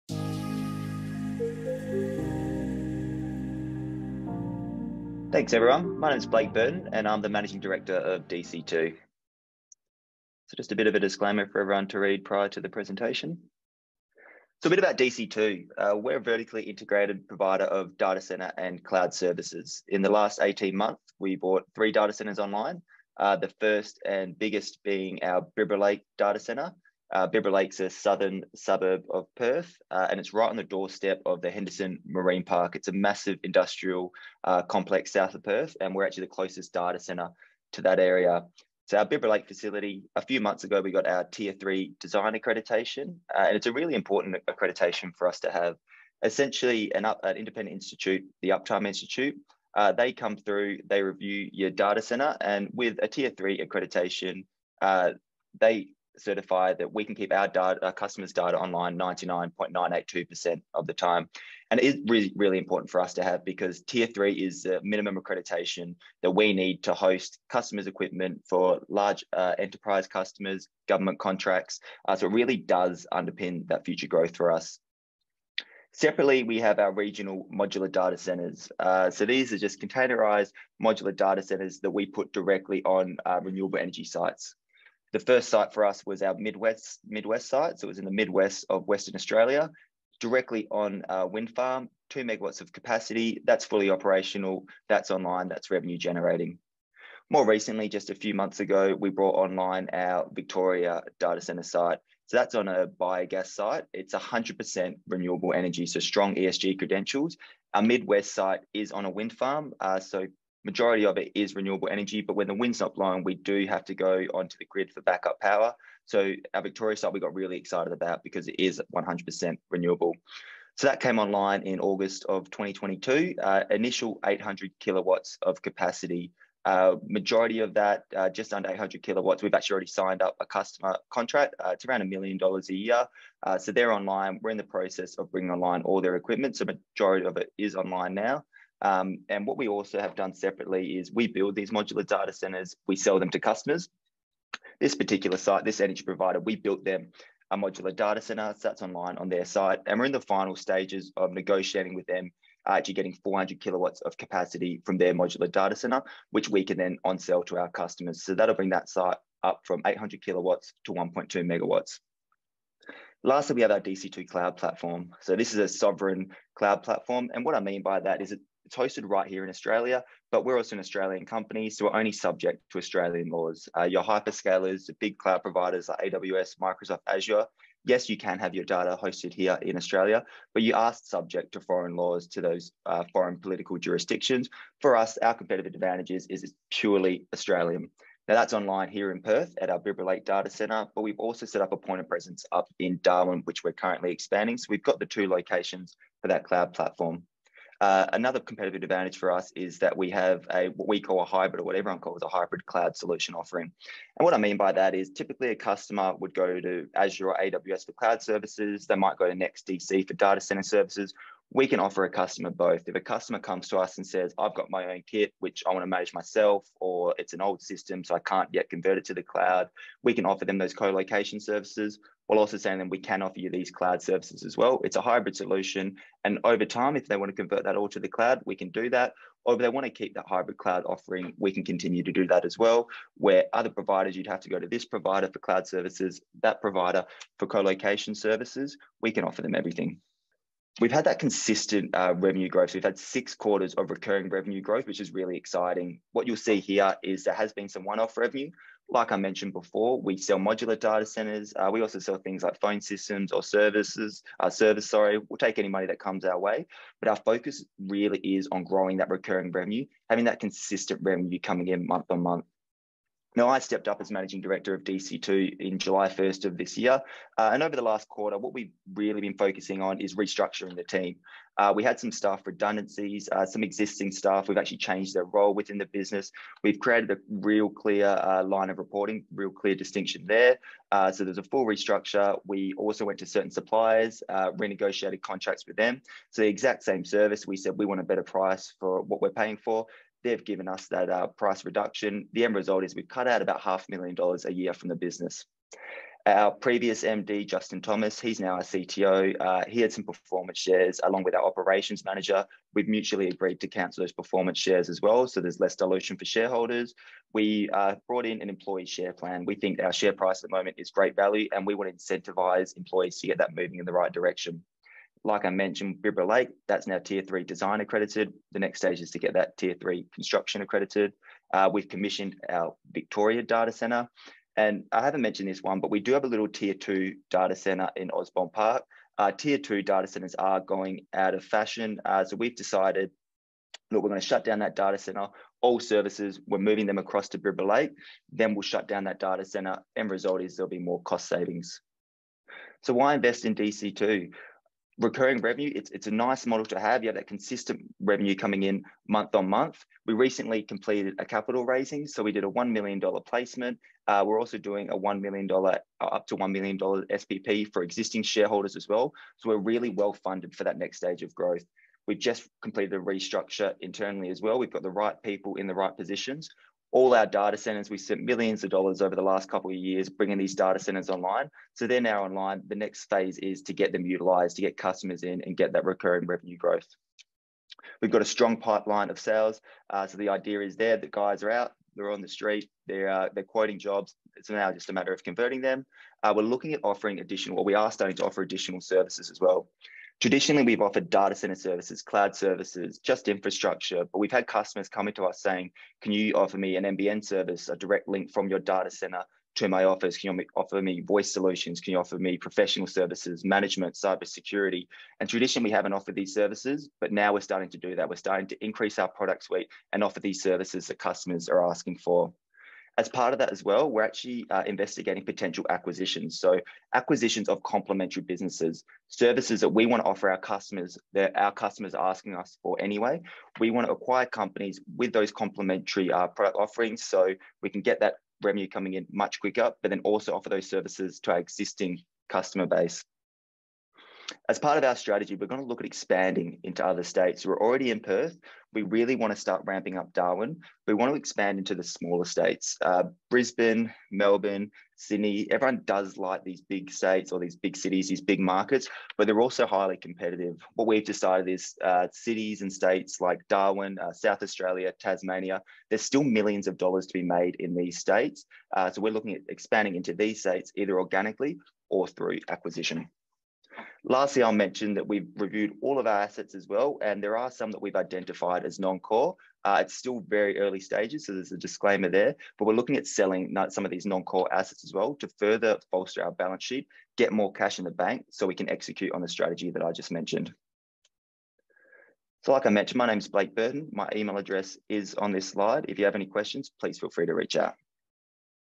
DC Two (ASX:DC2): Emerging ASX Gems presentation
November 2022: DC Two (ASX:DC2) Managing Director Blake Burton presents at The Capital Network's Emerging ASX Gems investment conference on Friday 4 November 2022.